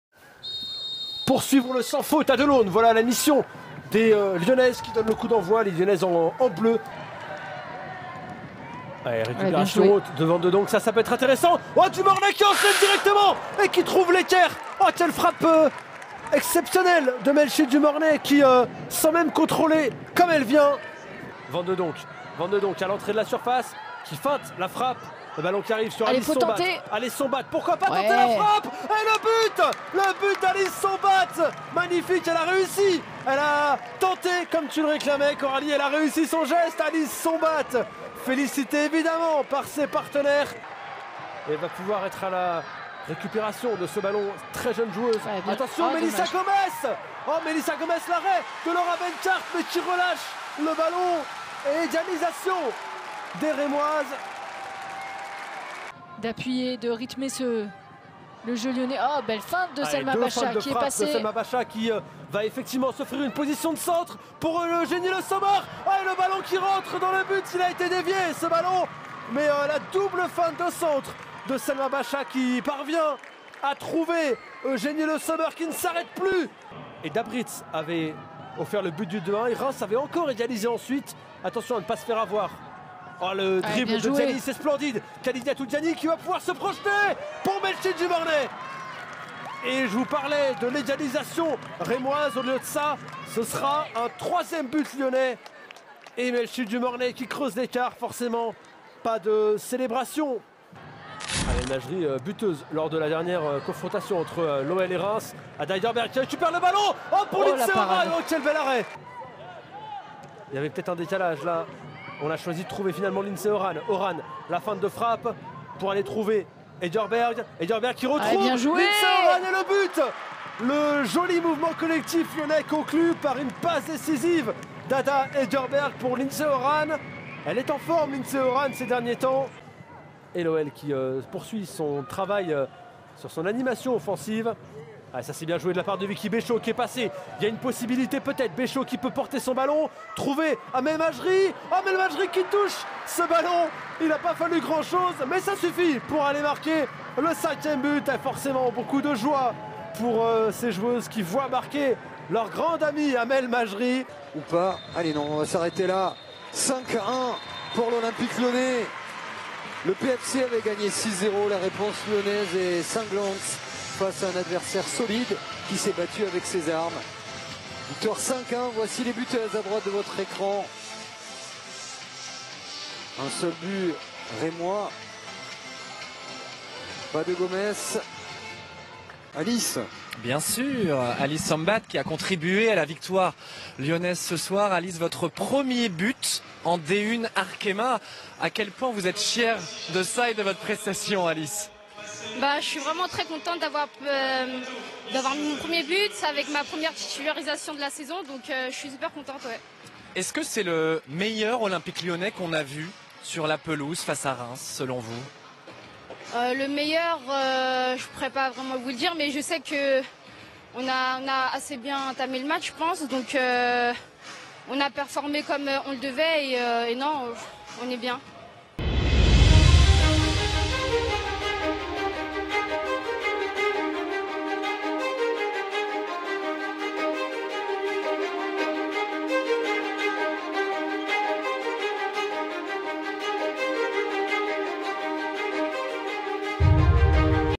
Lyon vs Reims Women's Division 1 Highlights | Match Day 5
Sonia Bompastor's players are undefeated this season. For this meeting the Lyon coach must do without Christiane Endler and Ellie Carpenter, both of whom have already left for the selection. Wendie Renard starts on the bench. The Lyonnaises are rewarded with the first goal in D1 Arkema from Alice Sombath on a heavy shot poorly negotiated by the goalkeeper.

The next meeting is set for November 5 and it's a shock since our Lyonnaises will travel to the field of Paris FC, co-leader of D1 Arkema.

Stade de Reims Women's LineUp :

Szemik - Ngock, Kack, Demehin, Pasquereau, Ndzana - Chossenotte (Eninger 61'), Joly (Corboz 61'), Menene (Le Moguédec 74'), Imuran (Mouchon 74') - Gomes (Nassi 79').

Coach : Amandine Miquel

Olympique Lyonnais Women's LineUp:

Benkarth - Sombath, Gilles, Mbock (C), Bacha - Damaris (Däbritz 60'), van de Donk (Hegerberg 46'), Horan - Diani (Becho 61'), Le Sommer D. (Morroni 65'), Dumornay Majri 65').

Coach : Sonia Bompastor

Goal :
Sombath (20'), Le Sommer (43'), Dumornay (45'), Horan (87 ') and Majri (90') for OL. Imuran (27') for Reims.

Referee : Clémence Goncalves